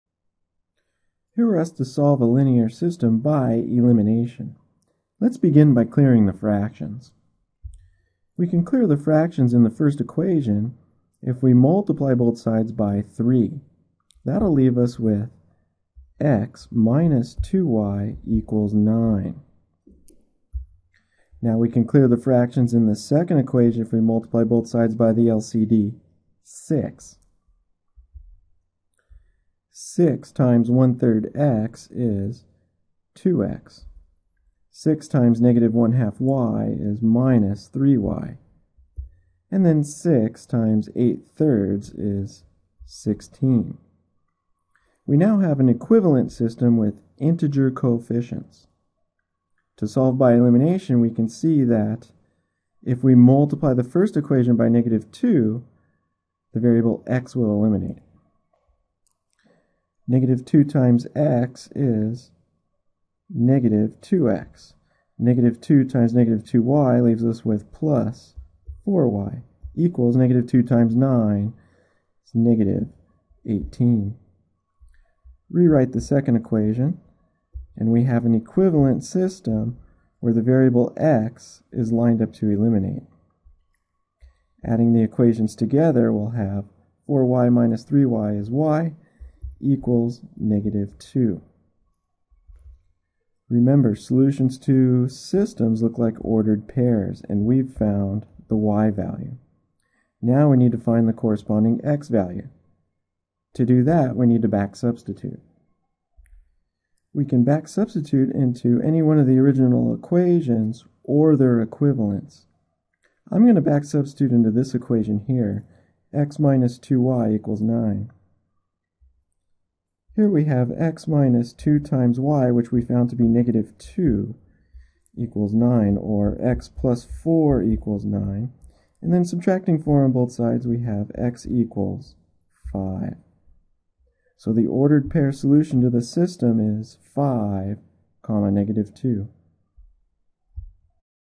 Solve a System by Elimination - Algebra fwk - TryThis04-0303
Solve a linear system with fraction coefficients by elimination. Solving Linear Systems by Elimination